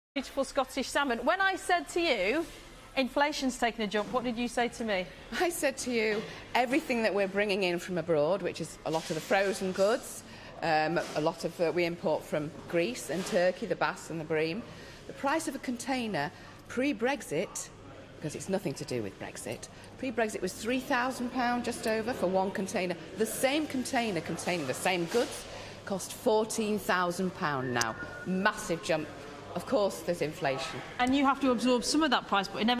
Price increase £3k to £14k "but 'it's nothing to do with Brexit'" says Mariella Gabutt to please BBC
Mariella Gabutt's critical comments about the effect of Brexit on her fish supply business were edited out of a second broadcast of the interview it conducted with her this week. Here Mariella gives some background to the situation that the BBC didn't want to broadcast